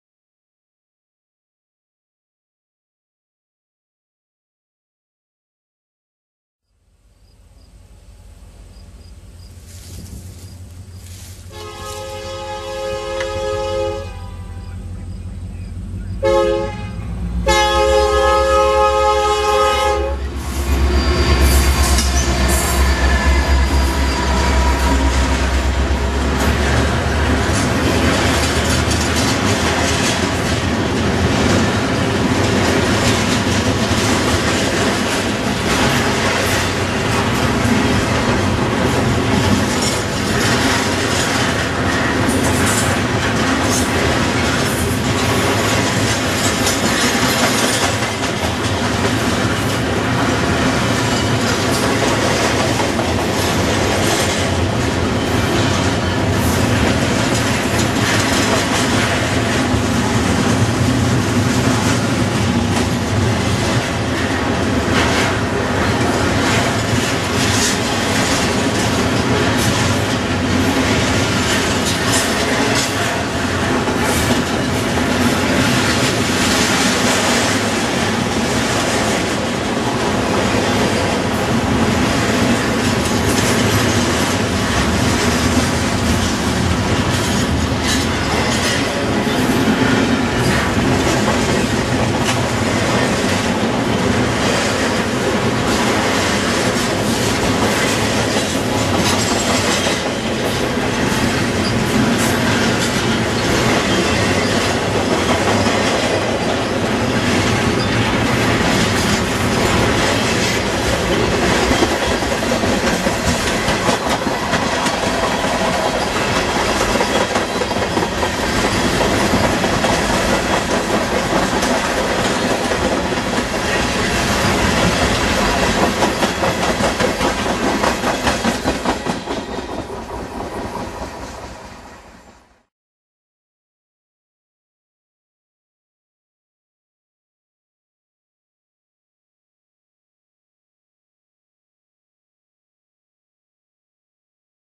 CSX R144 - Detour Train
CSX R144 heads east at Bannister Rd.